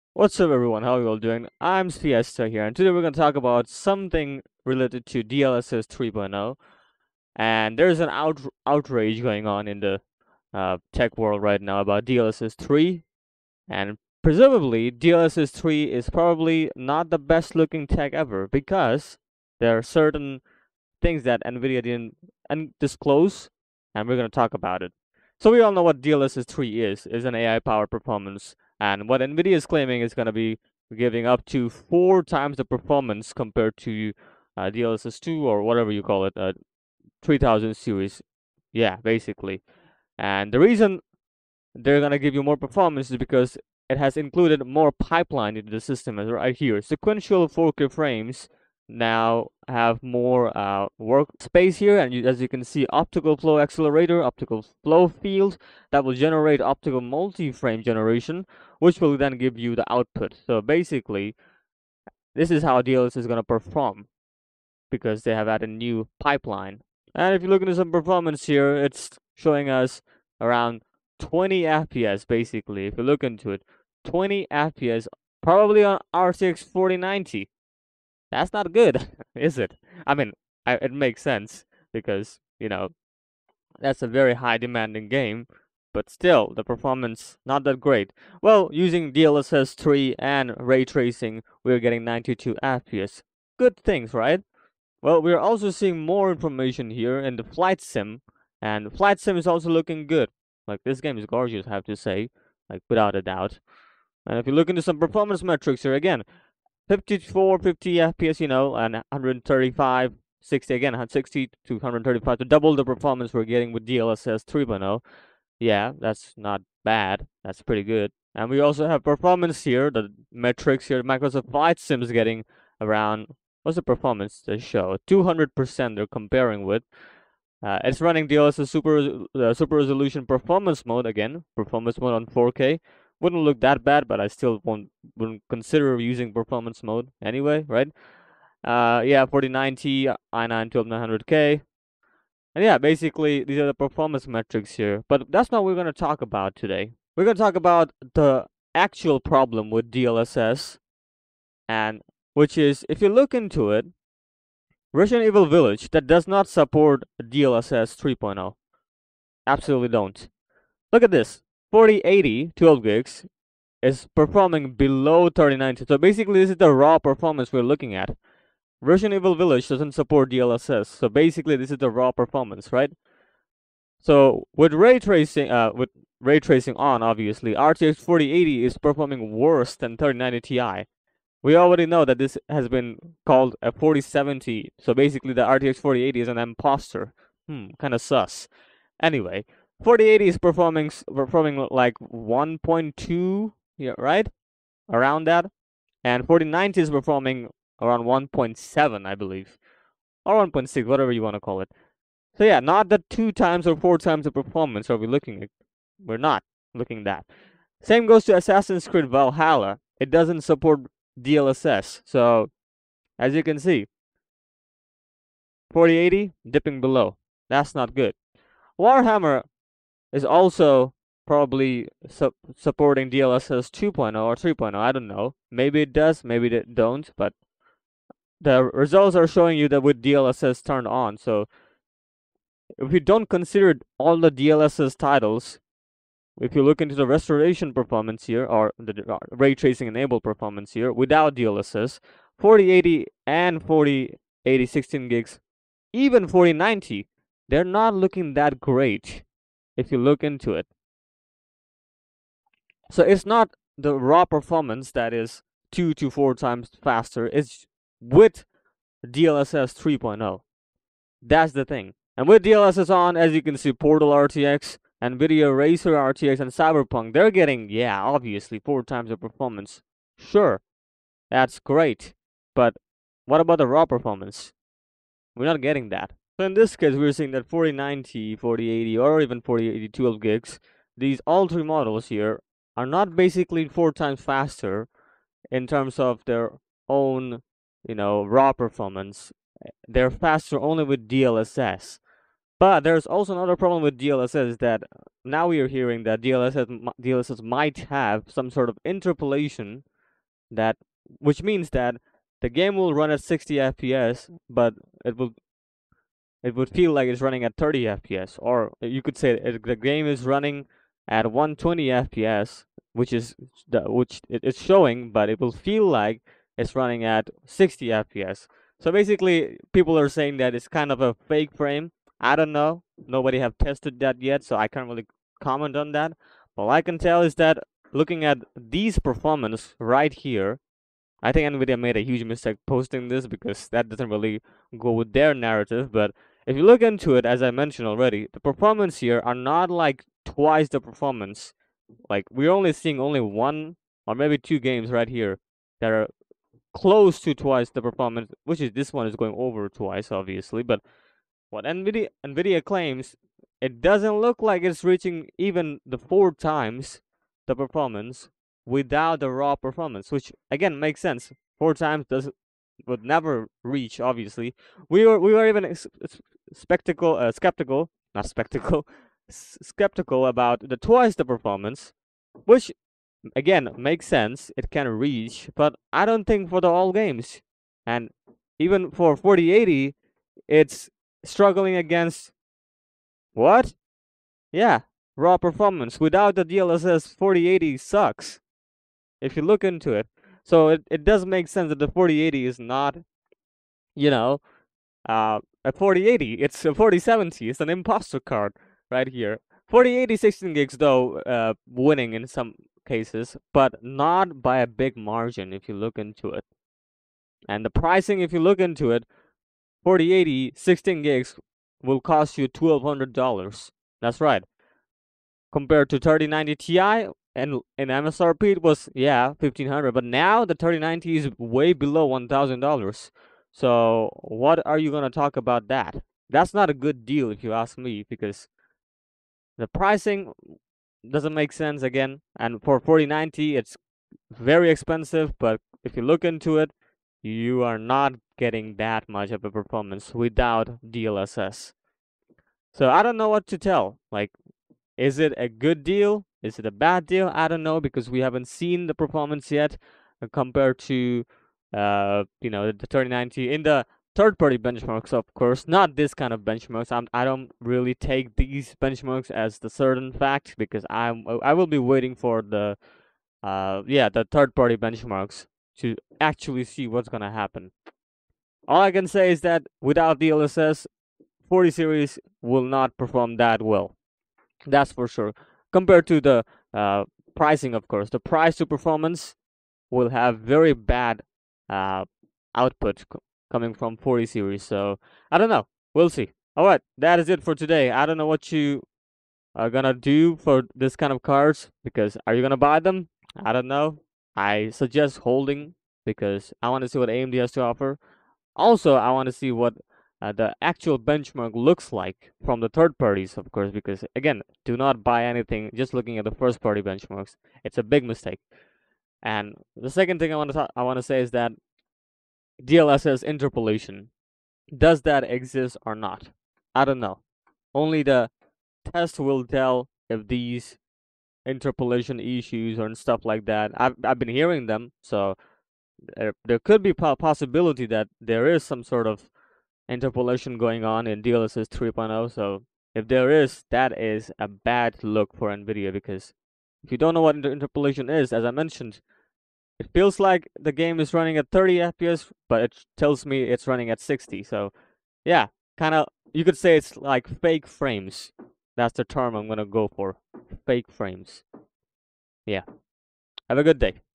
Is nvidia lying about RTX 4000 performance? Let's talk about it
In this video I talk about current RTX 4000 performance and DLSS 3.0 having some sort of interpolation.